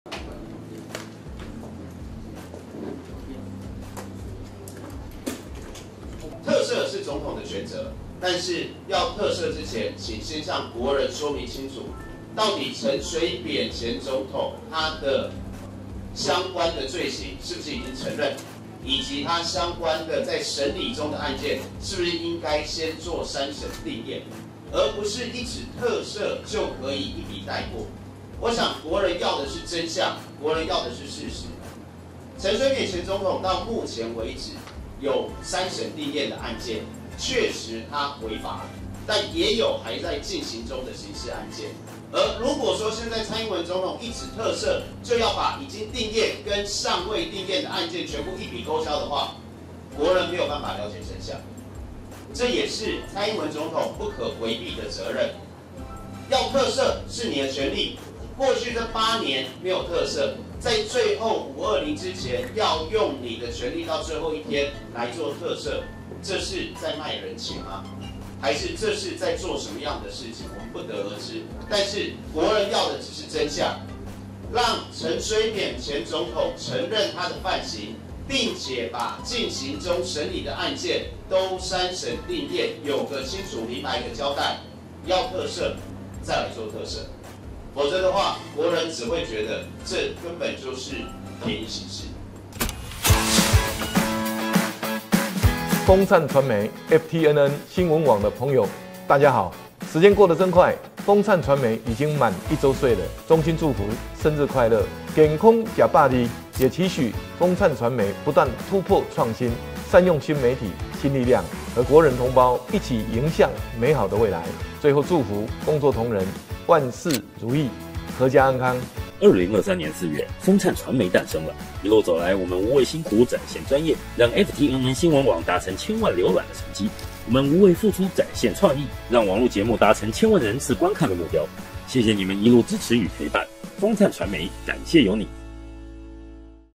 520前特赦陳水扁？國民黨團質疑在賣人情
記者陳思誼／台北報導

外傳總統蔡英文將在520卸任前特赦前總統陳水扁，國民黨立法院黨團書記長洪孟楷今(7)日表示，特赦是總統的權責，但特赦前請向國人說清楚，陳水扁相關罪刑是否已經承認，洪孟楷接著指出，蔡英文過去八年沒有特赦陳水扁，卻打算在總統權力行使至的最後一刻才特赦，質疑「這是在做人情嗎？ 還是做什麼樣的事情，外界不得而知。」